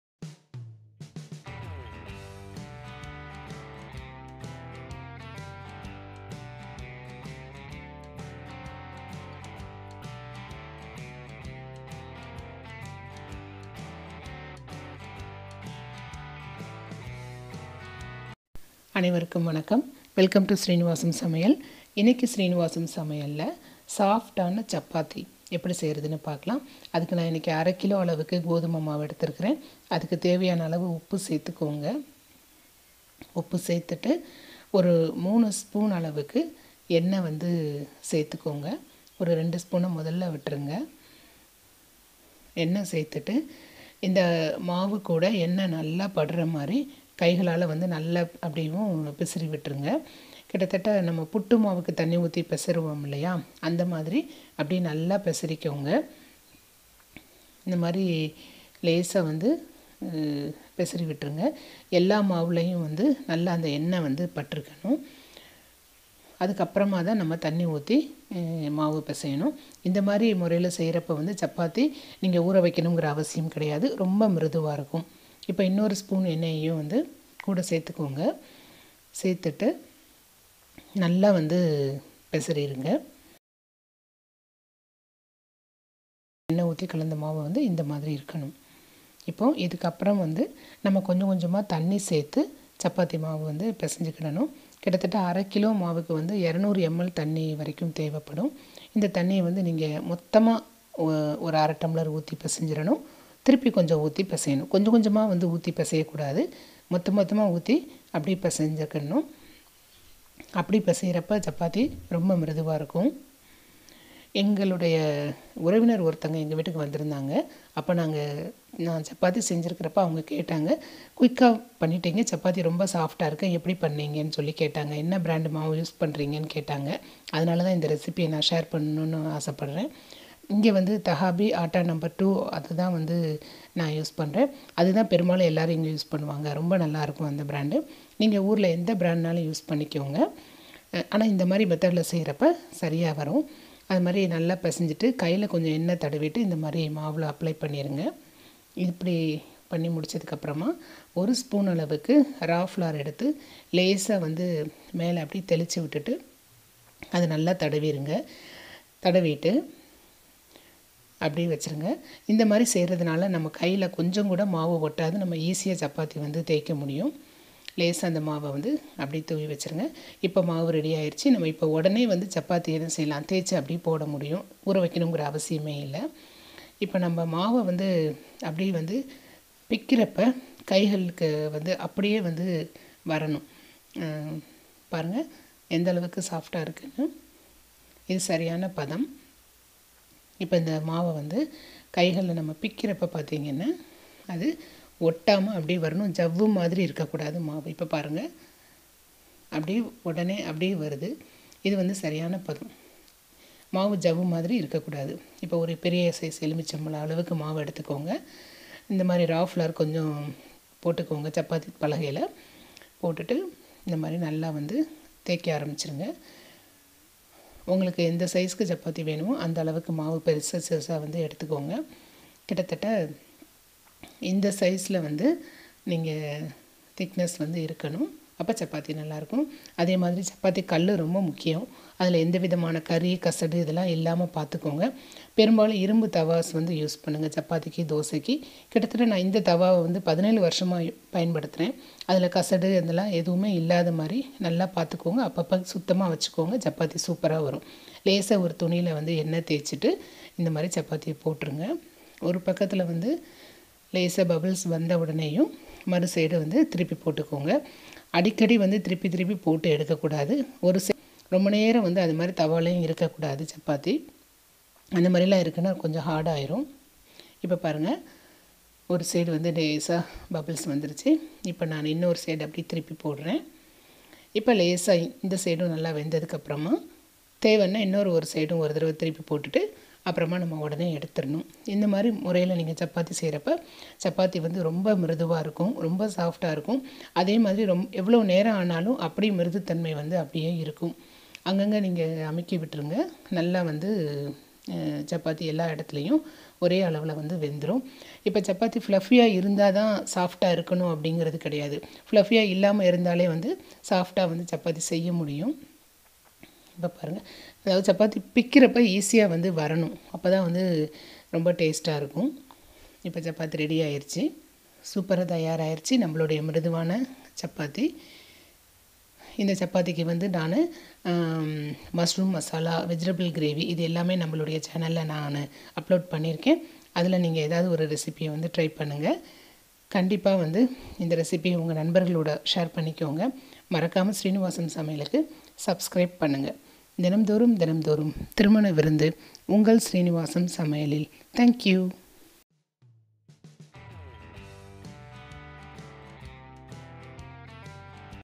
Soft Chappathi in Tamil | How to prepare Soft Chappathi in Tamil | Tasty,soft chappathi in Tamil
Hi everybody.Welcome to Srinivasam Samayal.Soft chappathi is easy to prepare without taking much time.All we need simple tips and tricks.Here is the recipe for you.Have a try and post your comments.
Ingredients:
Wheat flour_1/2kg
Water_250ml
Salt _to taste
Oil _3spoons
தினந்தோறும் தினந்தோறும் திருமண விருந்து.